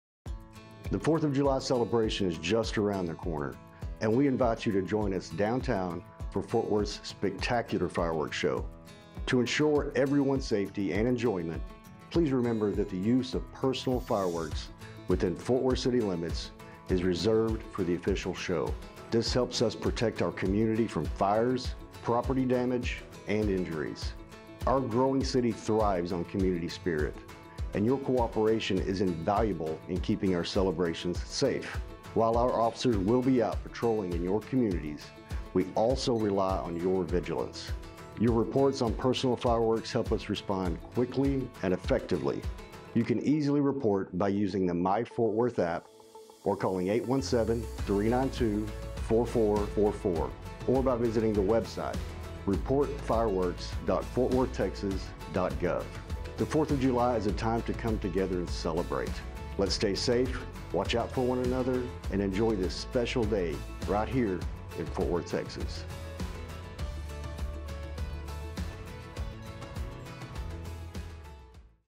City of Fort Worth | Fireworks Safety with Chief Noakes
As summer approaches, we have many reasons to celebrate in our community starting with July 4th.  We want to encourage you and your family to be safe and make good choices when you celebrate.

As a reminder, fireworks are illegal in Fort Worth.

They are dangerous because of the potential for damaged property and can also cause stress and anxiety to veterans, children and pets.

Besides being illegal, did you know that having or setting off fireworks can result in a fine of up to $2,000?

And remember, what goes up, must come down. Any celebratory gun shots can result in jail time.

Please do not call 9-1-1 to report fireworks.

Or report online using the MyFW App (Apple and Android available)

Download the FREE MyFW app on Apple store and Google Play Store.